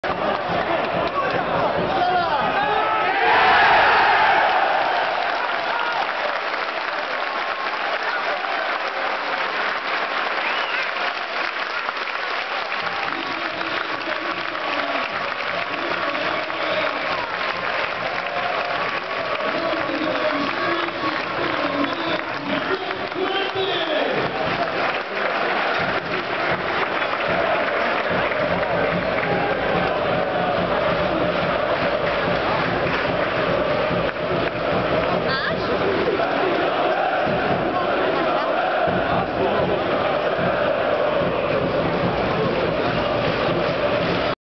2012.06.06 - Siarka Tarnobrzeg - Stal Kraśnik 6:1(4:0) 2-0 Jacek Kuranty 21'
Mecz decydujący o awansie do drugiej ligi.
Siarka Tarnobrzeg - Stal Kraśnik 6:1 (4:0) Bramka na 2-0 Jacek Kuranty 21'
Druga liga,druga liga SIARECZKA !!!